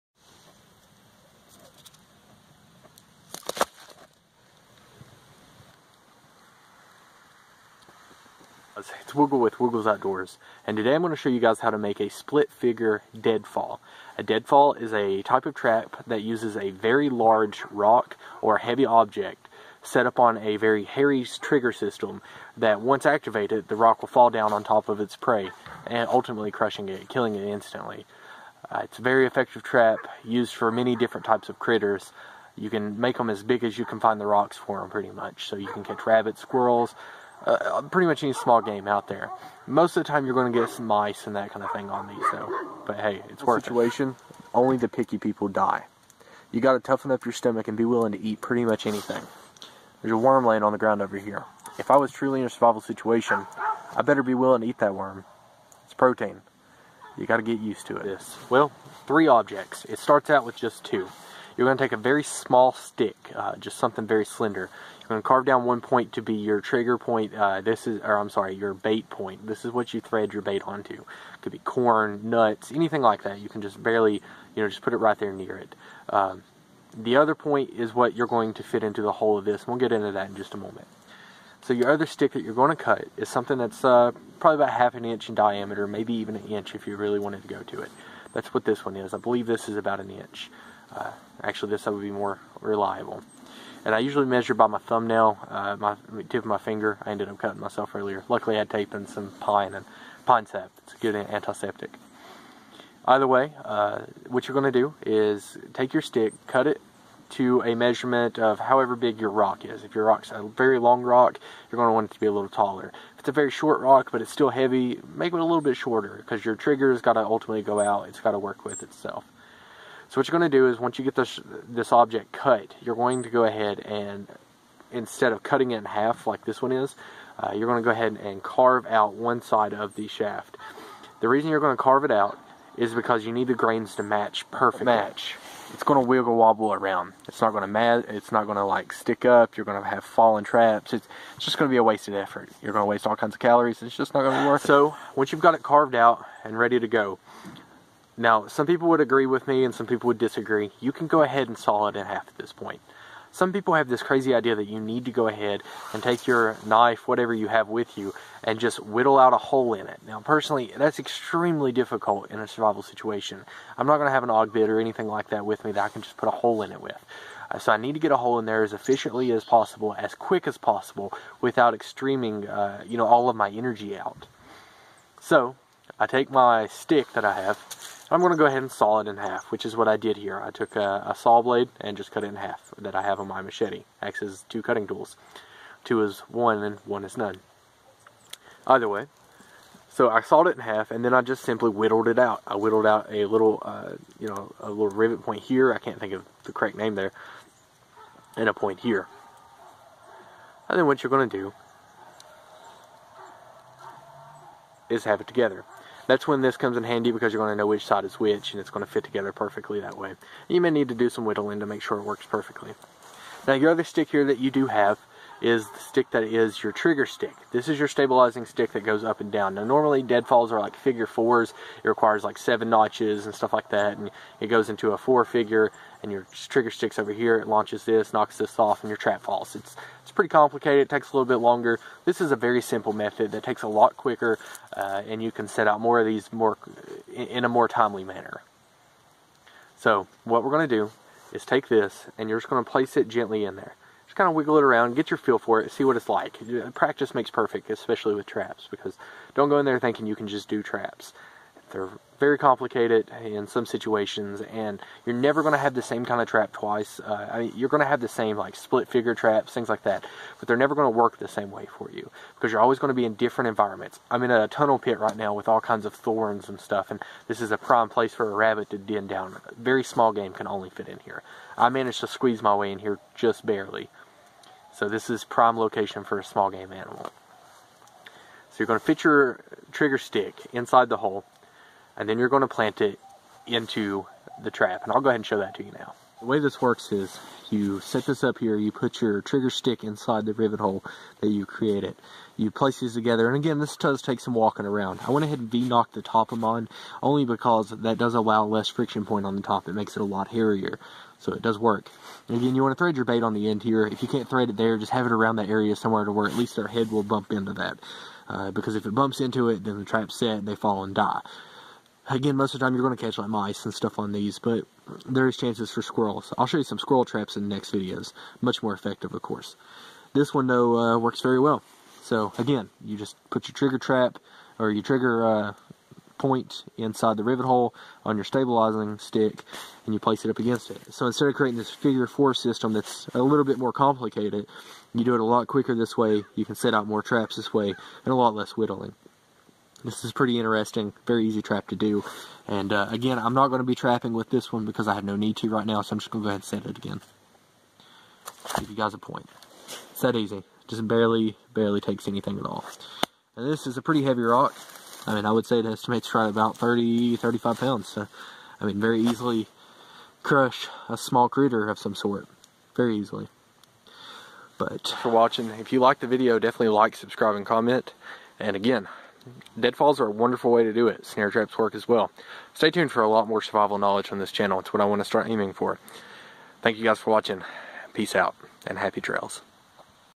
Split Stick Deadfall trap!: Self Reliance
A deadfall is a survival trap that works on a very heavy object set on a trigger system that when set will fall and kill whatever prey is underneath. It is designed for small game such as rabbits, squirrel, and mice. Most Deadfalls require 7 figure notches and other various notches. This one is rather simple and is easy to put out many in a short amount of time. It only requires two sticks and a rock. I hope you found this useful!
  Always remember with traps, watching is one thing actually practicing is another. Traps will always vary and require a personal touch to make them work in a more effective manner!